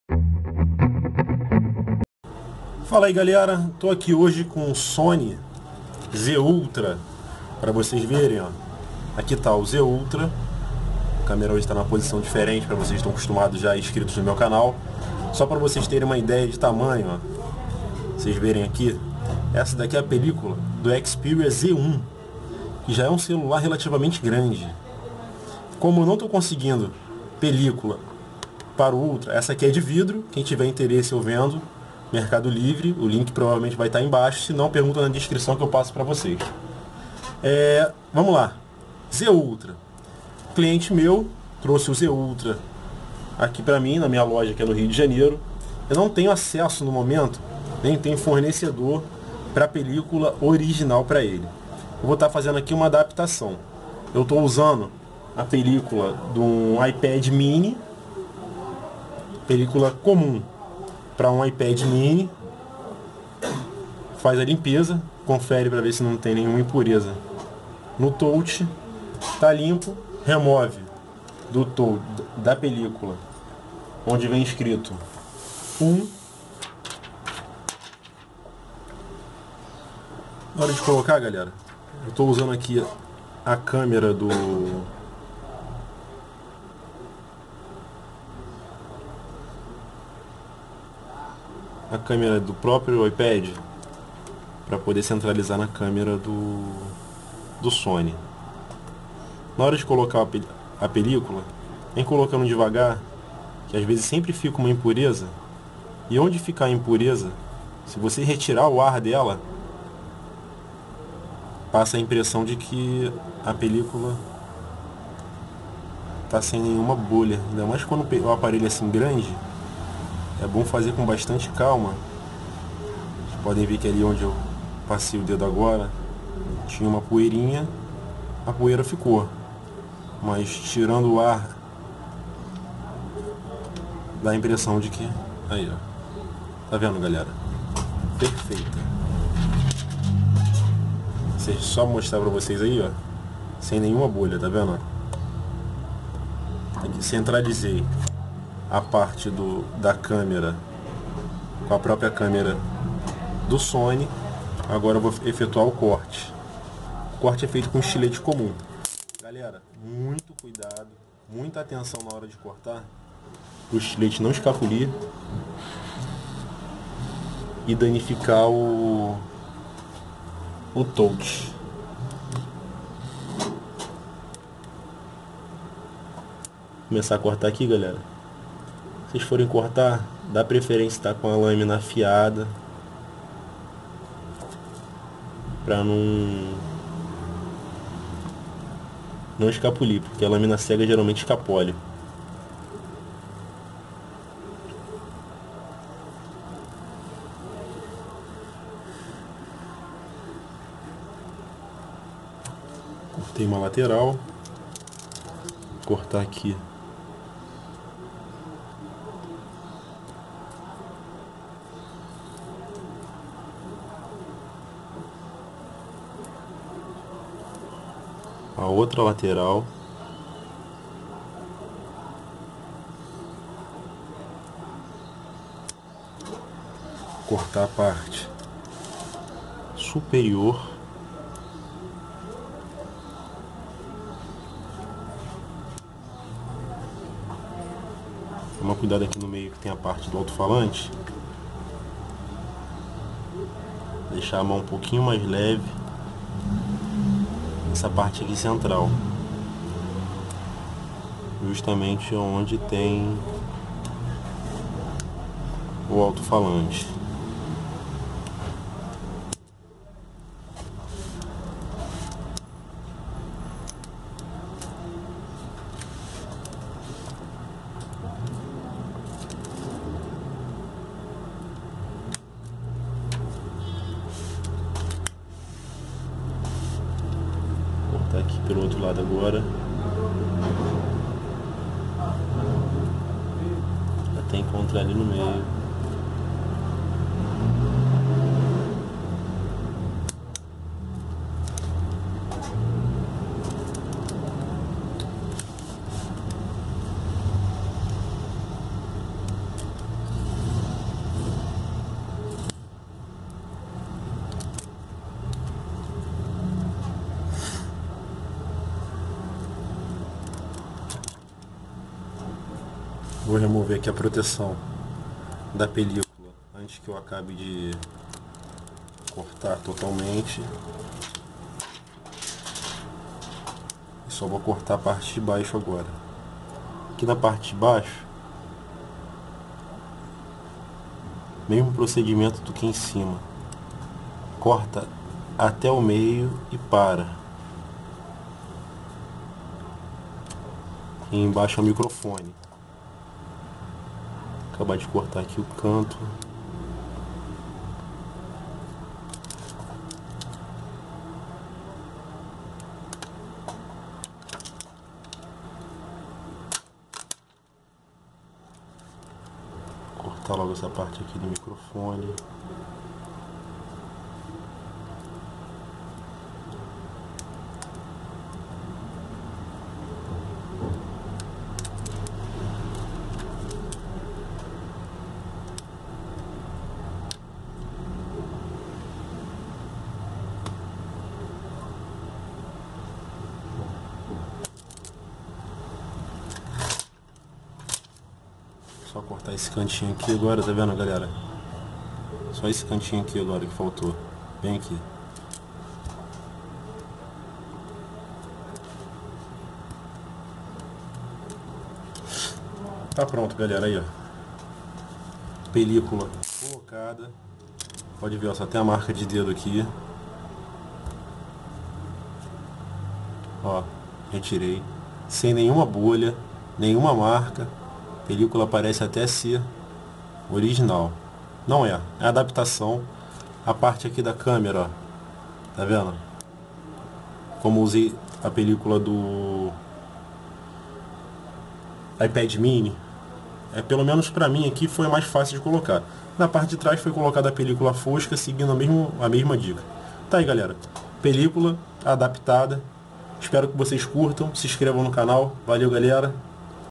Como colocar película Xperia Z Ultra
Como colocar película em seu celular ou tablet adaptada com praticamente qualquer película e um estilete.

How to put film on your phone or tablet adapted to almost any film and a stylus .